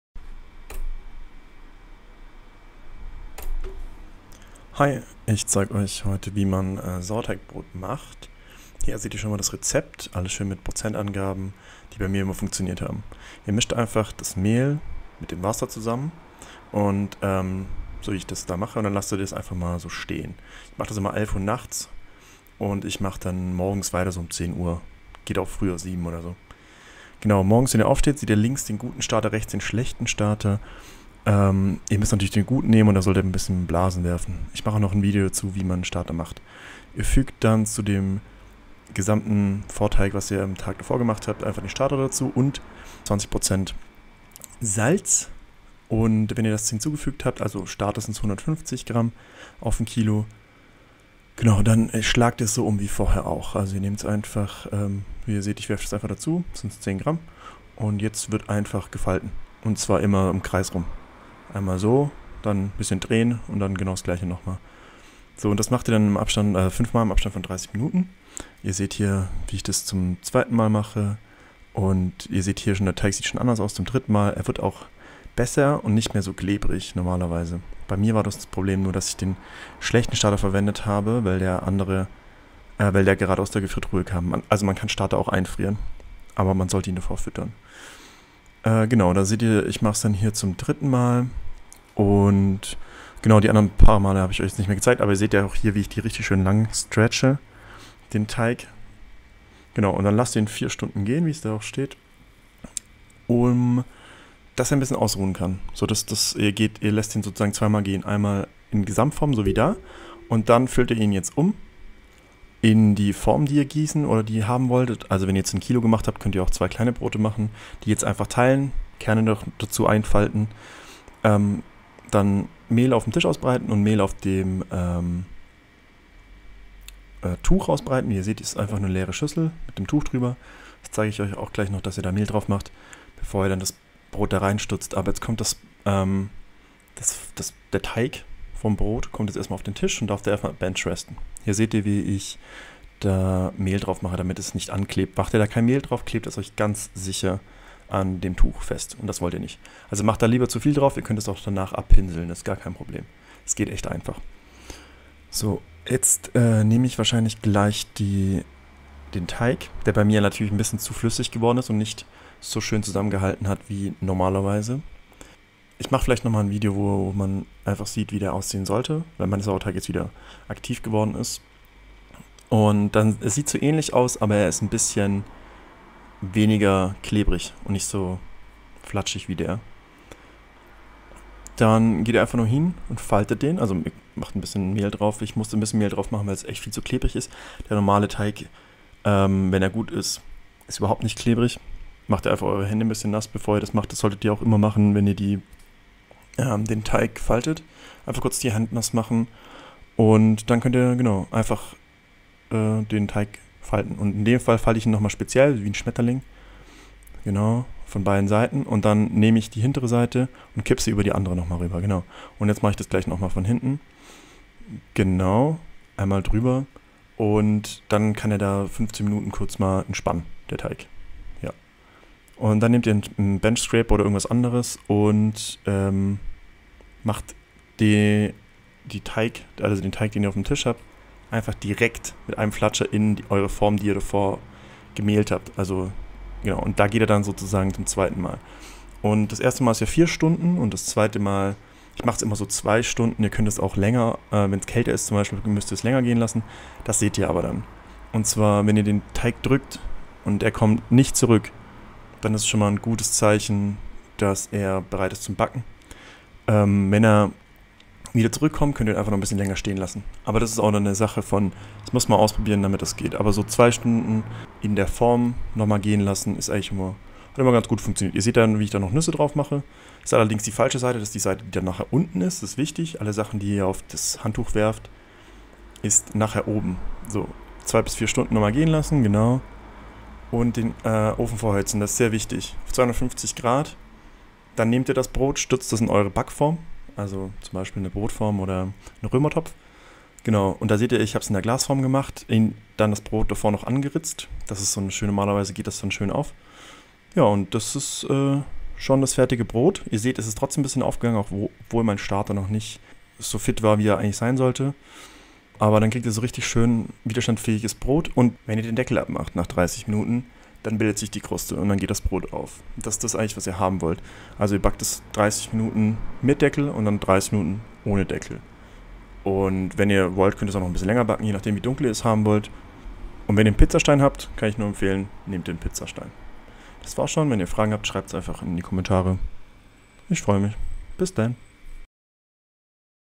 Sauerteigbrot backen
Ich erkläre euch kurz, wie man Sauerteig Brot macht, wie meine Zutaten Verhältnisse sind und auf was machen achten sollte.